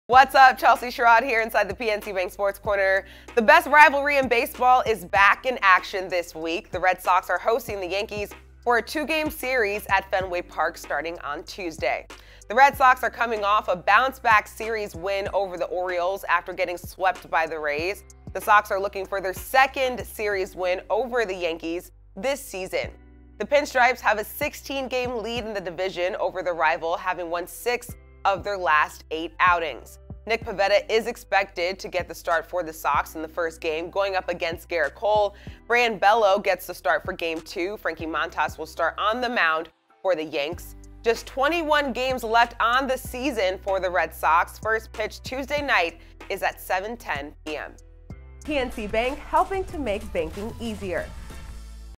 Red Sox Host Rival Yankees For Two-Game Series At Fenway Park
The best rivalry in baseball is back at Fenway Park Tuesday night as the Red Sox host the Yankees for a two-game series.